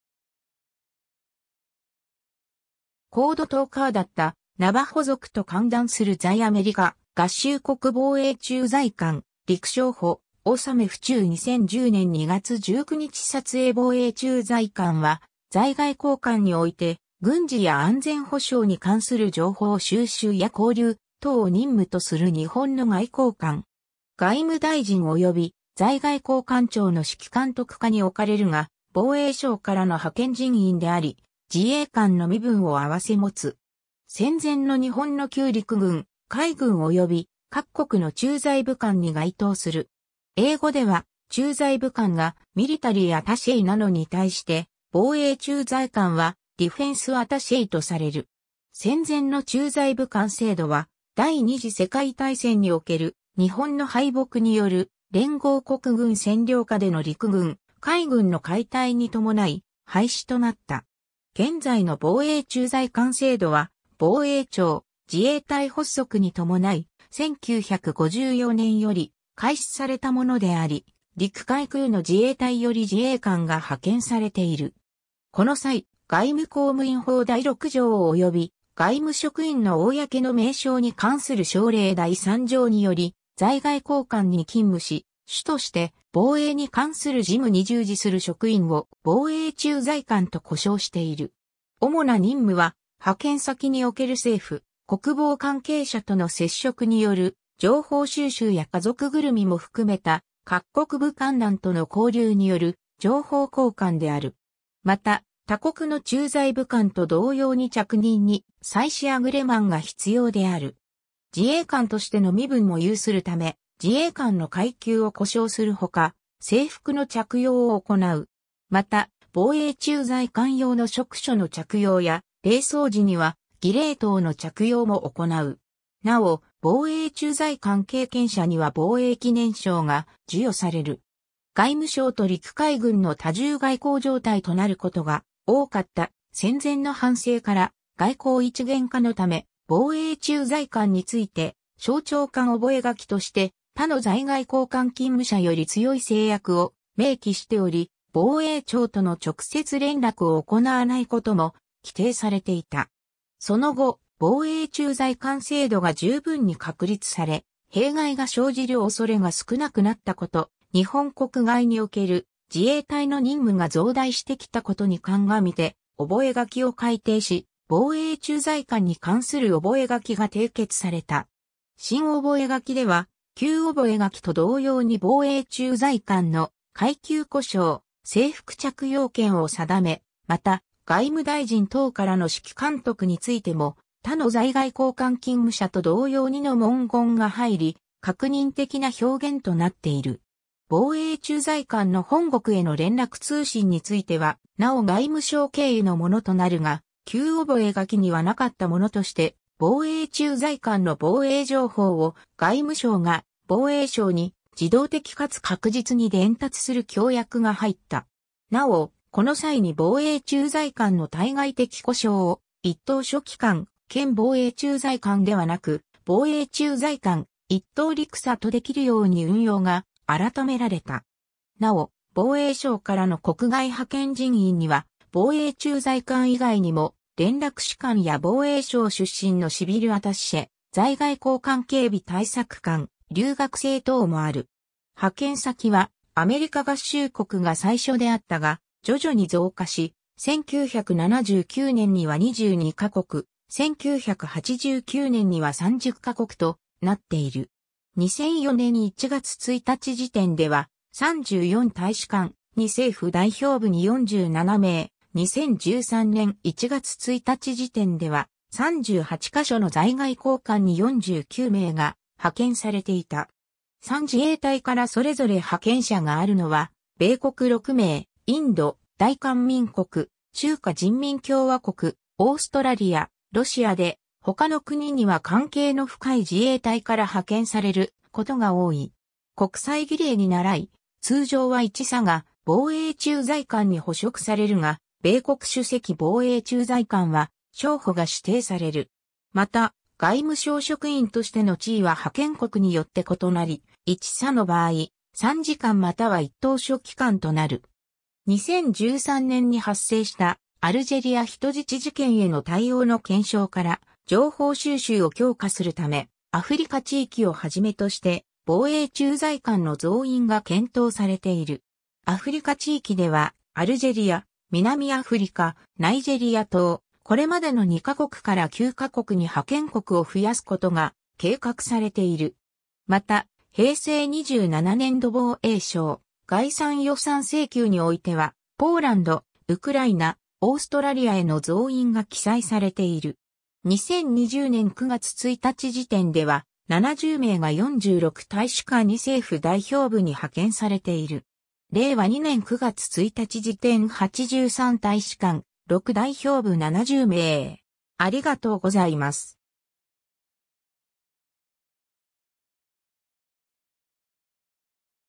防衛駐在官
日本の諜報・情報機関
コードトーカーだったナバホ族と歓談する在アメリカ合衆国防衛駐在官：陸将補 納富中 2010年（平成22年）2月19日撮影 防衛駐在官（ぼうえいちゅうざいかん）は、在外公館において軍事や安全保障に関する情報収集や交流等を任務とする日本の外交官（外務事務官）。
外務大臣及び在外公館長の指揮監督下に置かれるが、防衛省からの派遣人員であり、自衛官の身分を併せ持つ。
戦前の日本の旧陸軍・海軍及び各国の駐在武官に該当する。
英語では、駐在武官が Military Attaché なのに対して防衛駐在官は Defense Attaché とされる。
 戦前の駐在武官制度は、第二次世界大戦における日本の敗北による連合国軍占領下での陸軍・海軍の解体に伴い、廃止となった。
現在の防衛駐在官制度は、防衛庁・自衛隊発足に伴い、1954年（昭和29年）より開始されたものであり、陸海空の自衛隊より自衛官が派遣されている。
この際、外務公務員法第6条及び外務職員の公の名称に関する省令第3条により「在外公館に勤務し、主として防衛に関する事務に従事する職員」を防衛駐在官と呼称している。
主な任務は、派遣先における政府・国防関係者との接触による情報収集や家族ぐるみも含めた各国武官団との交流による情報交換である。
また、他国の駐在武官と同様に着任に際しアグレマンが必要である。
 自衛官としての身分も有するため、自衛官の階級を呼称するほか、制服の着用を行う。
また、防衛駐在官用の飾緒の着用や、礼装時には、儀礼刀の着用も行う。
なお、防衛駐在官経験者には防衛記念章（外国勤務経験者）が授与される。
 外務省と陸海軍の多重外交状態となることが多かった戦前の反省から、外交一元化のため、防衛駐在官について、省庁間覚書（「防衛庁出身在外公館勤務者の身分等に関する外務事務次官、防衛庁次長覚書」（昭和30年8月8日））として、他の在外公館勤務者より強い制約を明記しており、防衛庁との直接連絡を行わないことも規定されていた。
その後、防衛駐在官制度が十分に確立され、弊害が生じるおそれが少なくなったこと、日本国外における自衛隊の任務が増大してきたことに鑑みて、覚書を改定し、「防衛駐在官に関する覚書」（平成15年5月7日）が締結された。
 新覚書では、旧覚書と同様に防衛駐在官の階級呼称・制服着用権を定め、また外務大臣等からの指揮監督についても「他の在外公館勤務者と同様に」の文言が入り確認的な表現となっている。
防衛駐在官の本国への連絡通信についてはなお外務省経由のものとなるが、旧覚書にはなかったものとして防衛駐在官の防衛情報を外務省が防衛省に自動的かつ確実に伝達する協約が入った。
なお、この際に防衛駐在官の対外的呼称を「一等書記官（又は参事官）兼防衛駐在官」ではなく、「防衛駐在官・1等陸（又は海・空）佐」とできるように運用が改められた。
なお、防衛省からの国外派遣人員には、防衛駐在官以外にも、連絡士官や防衛省出身のシビル・アタッシェ（自衛官以外の防衛省職員・いわゆる背広組）、在外公館警備対策官、留学生等もある。
 派遣先はアメリカ合衆国（在米大使館）が最初であったが、徐々に増加し、1979年（昭和54年）には22ヶ国、1989年（昭和64年/平成元年）には30ヶ国となっている。
2004年（平成16年）1月1日時点では、34大使館2政府代表部に47名（うち陸22名・海13名・空12名）、2013年（平成25年）1月1日時点では、38カ所の在外公館に49名（陸23名・海13名・空13名）が派遣されていた。
三自衛隊からそれぞれ派遣者があるのは米国6名（陸海空各2名）、インド、大韓民国、中華人民共和国、オーストラリア、ロシア（陸海空各1名）で、他の国には関係の深い自衛隊から派遣されることが多い。
国際儀礼にならい、通常は1佐（三）（大佐相当）が防衛駐在官に補職されるが、米国首席防衛駐在官は将補（二）（少将相当）が指定される。
また、外務省職員（外交官）としての地位は派遣国によって異なり、1佐の場合、参事官（主要国のみ）又は1等書記官となる。
 2013年（平成25年）に発生したアルジェリア人質事件への対応の検証から、情報収集を強化するため、アフリカ地域をはじめとして防衛駐在官の増員が検討されている。
アフリカ地域では、アルジェリア、南アフリカ、ナイジェリア等、これまでの2ヶ国から9ヶ国に派遣国を増やすことが計画されている。
また、平成27年度防衛省概算予算請求においては、...